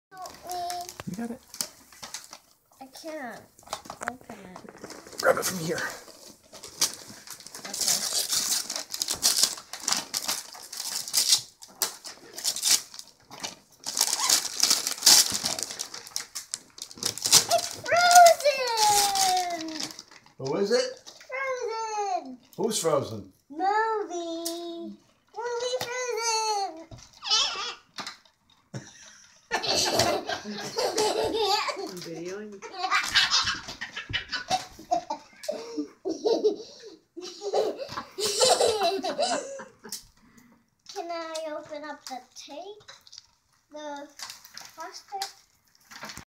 Someone is a bit excited about Frozen...
Lilly opening one of her presents.